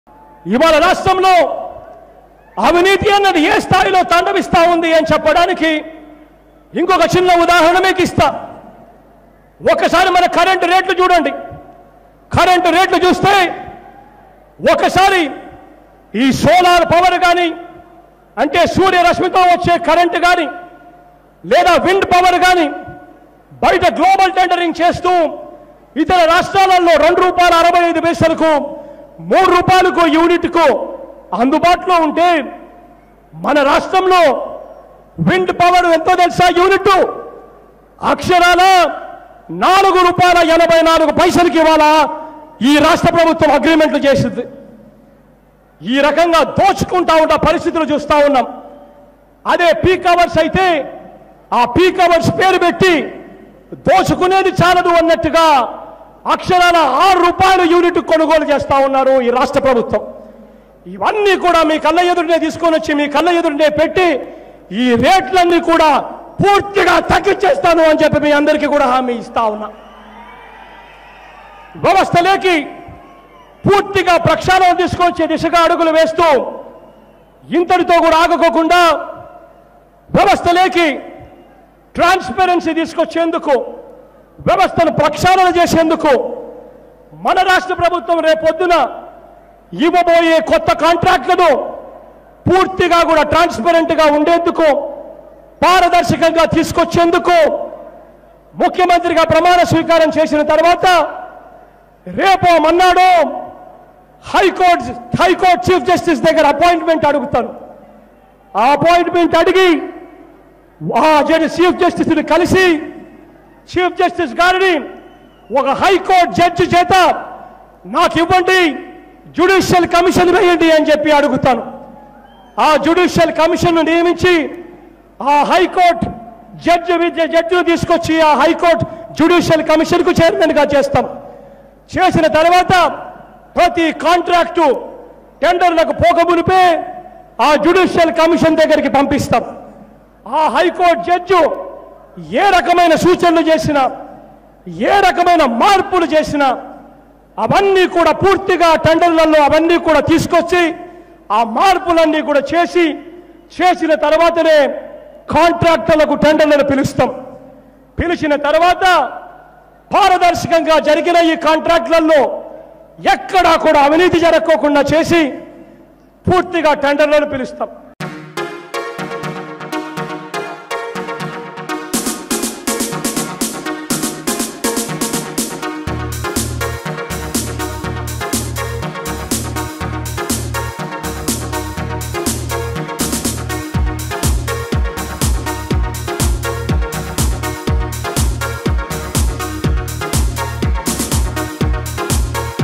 టీడీపీ అవినీతిపై జగన్ షాకింగ్ కామెంట్స్| CM YS Jagan Shocking comments on TDP Leaders Corruption
Watch : YS Jagan Speech about TDP Leaders and Janmabhumi Committees Corruption
Amaravathi Media